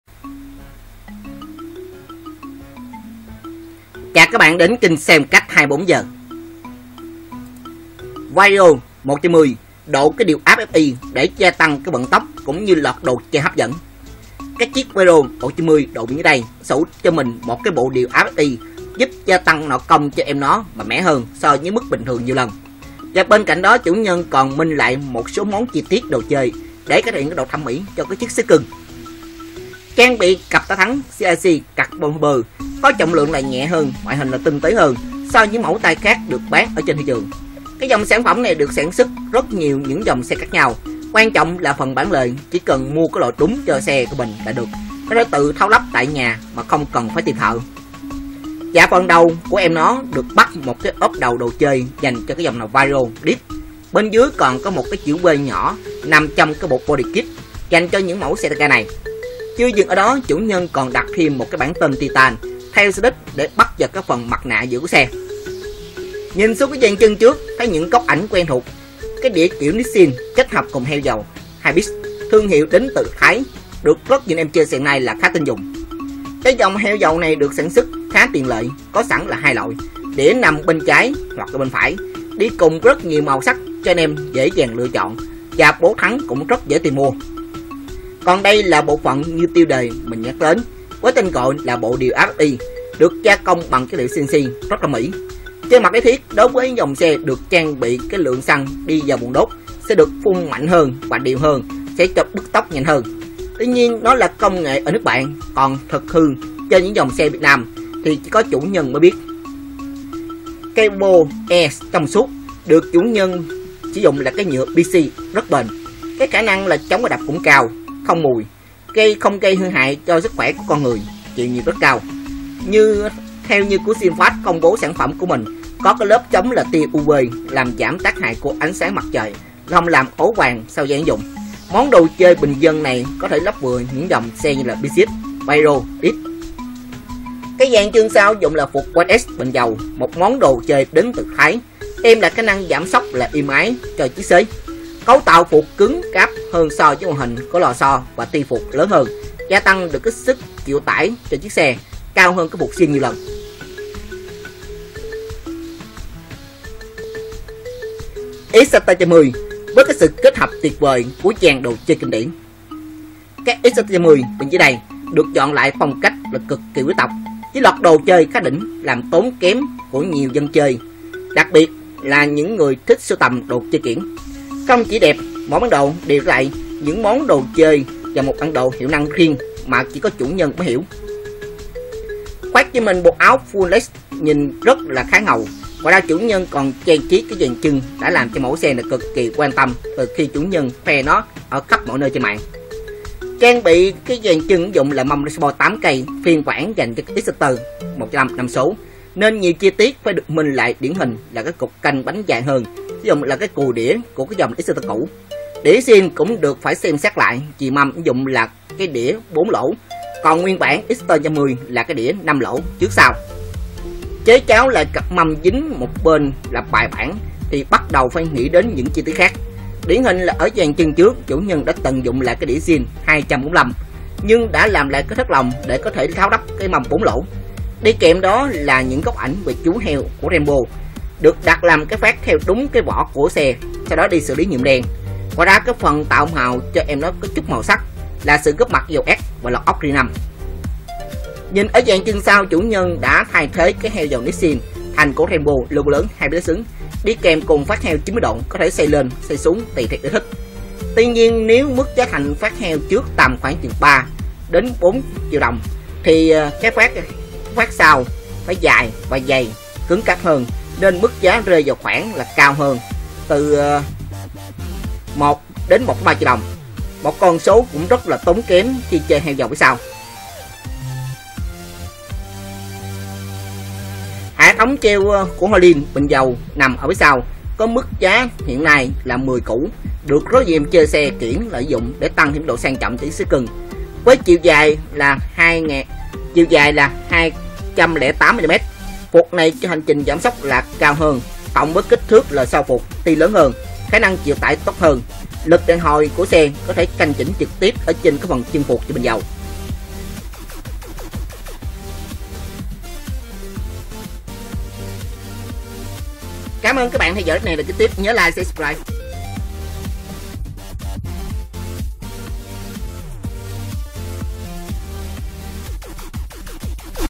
Xe & Phong Cách 24h - Vario 150 độ bộ điều áp Fi để gia tăng vận tốc cùng loạt đồ chơi hấp dẫn.
Vario 150 độ dưới đây đang sở hữu thêm cho mình bộ điều áp Fi giúp gia tăng nội công cho xe mạnh hơn bình thường lên nhiều lần. Bên cạnh đó chủ nhân của nó còn mod lại một số chi tiết đồ chơi để cải thiện độ thẩm mỹ cho chiếc xe.